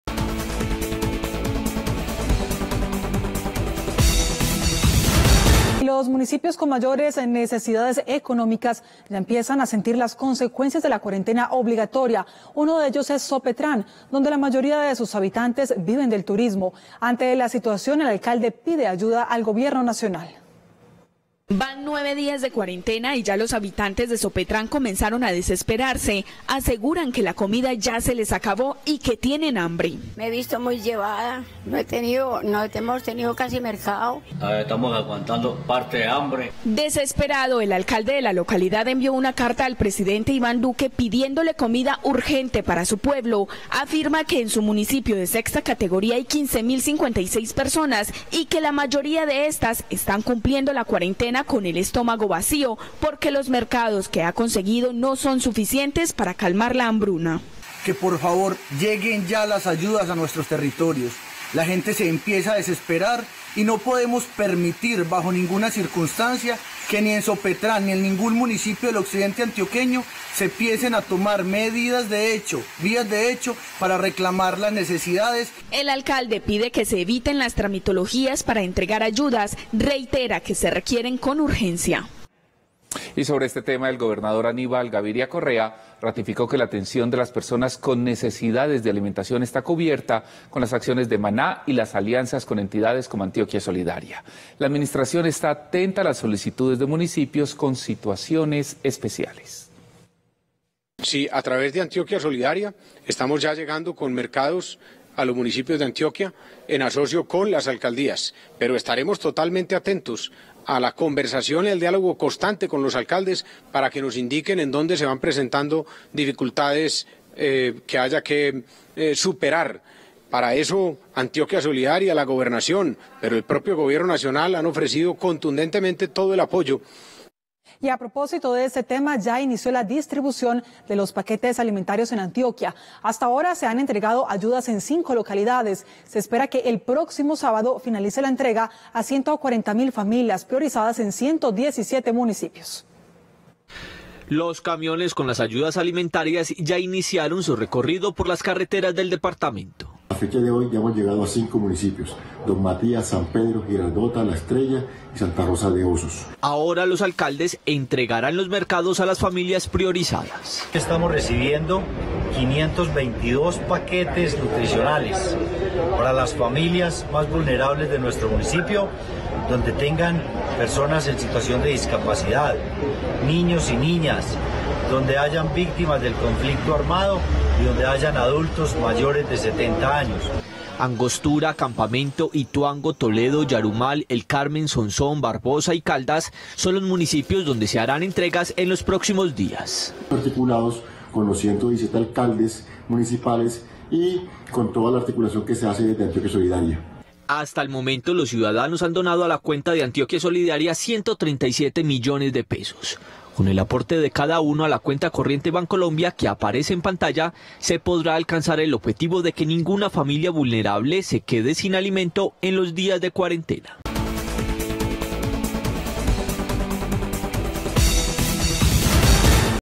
Comunidad en Sopetrán clama por alimentos durante la cuarentena - Teleantioquia Noticias
Las consecuencias de la cuarentena obligatoria ya se empiezan a sentir en los municipios con mayores carencias económicas. Uno de ellos es Sopetrán, en el Occidente antioqueño,  donde los habitantes, que en su mayoría viven del turismo y actividades del día a día, dicen tener sus neveras vacías. Ante la situación, el alcalde de la localidad pide al Gobierno Nacional ayuda para su gente.